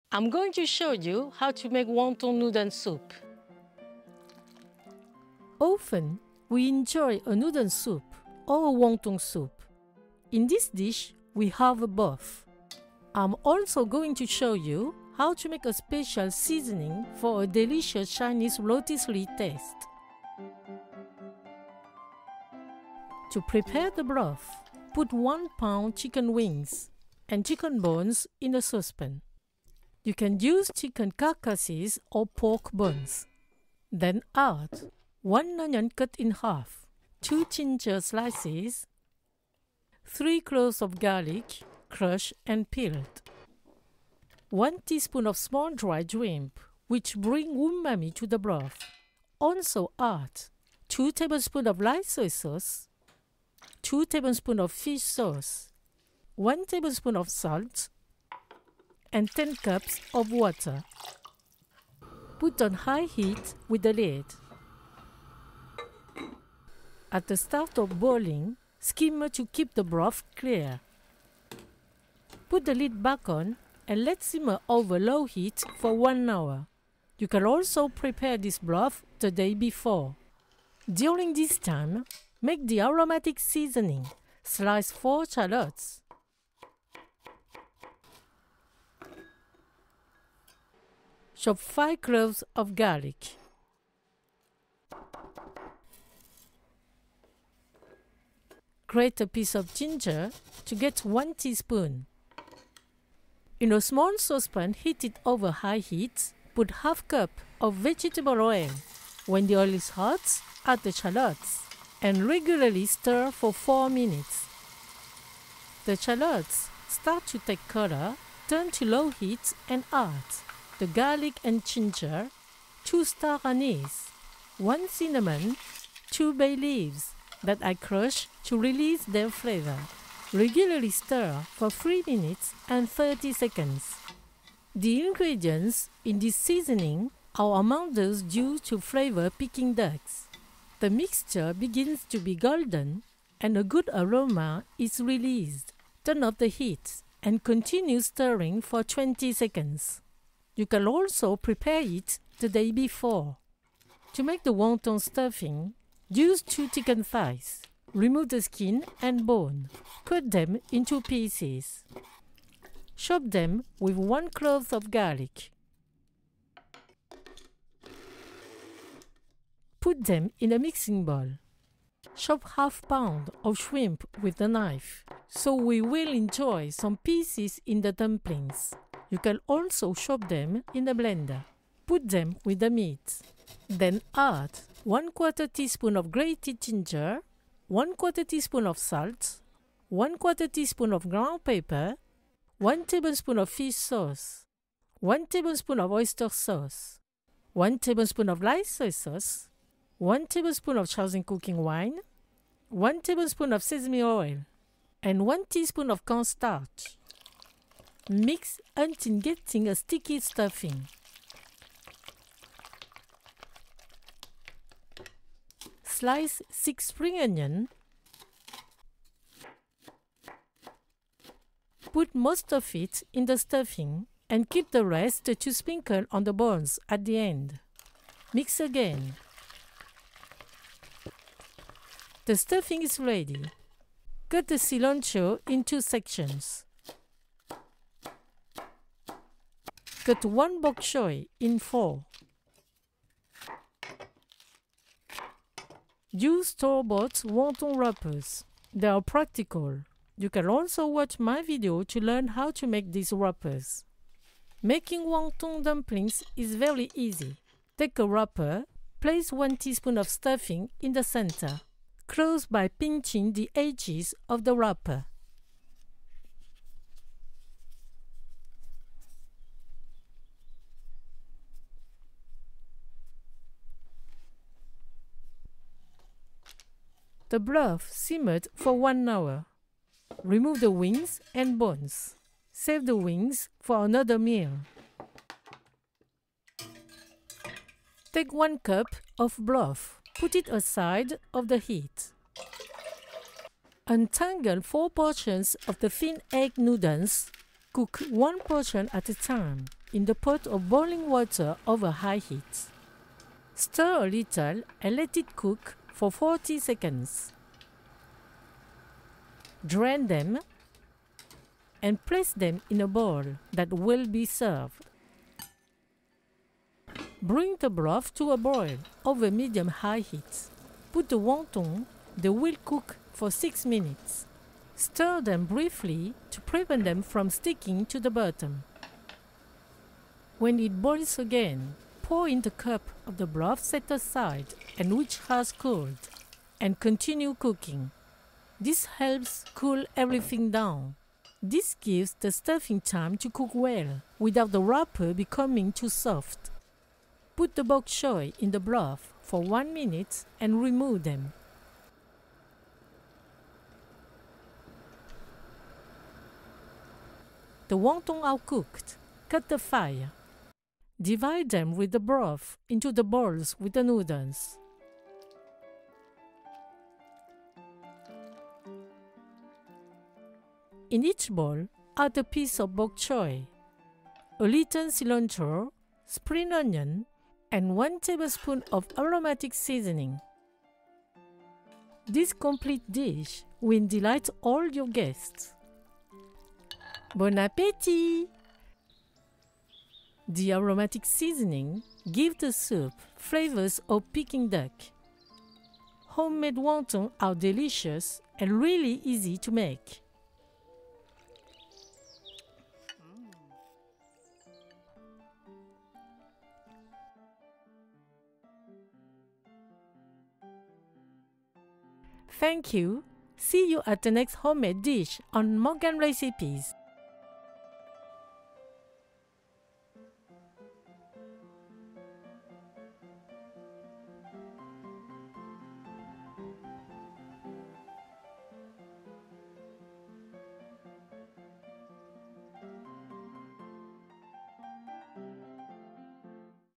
Chicken Wonton Noodle Soup, flavored with a very special fried aromatic seasoning. Easy to make!
Often we enjoy a noodle soup or a wonton soup, in this dish, we have both. I’m also going to show you how to make a special seasoning for a delicious “Chinese rotisserie” taste.

👩‍🍳 Ingredients for 4 people :

4 portions (6 oz) thin fresh egg noodles (or dehydrated)
2 spring onions
1 bok choy
2 tbsp cilantro

Broth:
1 lb chicken wings and bones
1 onion
2 slices of ginger
3 garlic cloves
1 tsp small dried shrimp
2 tbsp light soy sauce
2 tbsp fish sauce
1 tbsp salt
10 cups water (2.5 liters)

Wonton dumplings (approximately 30 pieces):
2 chicken thighs
½ lb frozen raw shrimp
1 garlic clove
¼ tsp grated ginger
¼ tsp salt
¼ tsp ground pepper
1 tbsp fish sauce
1 tbsp oyster sauce
1 tbsp light soy sauce
1 tbsp Shaoxing cooking wine
1 tbsp sesame oil
1 tsp cornstarch
4 spring onions
30 wonton wrapers

Aromatic seasoning:
5 garlic cloves
4 shallots
2 bay leaves
2 star anise
1 cinnamon stick
1 tsp grated ginger
½ cup vegetable oil

🎵 Music 🎵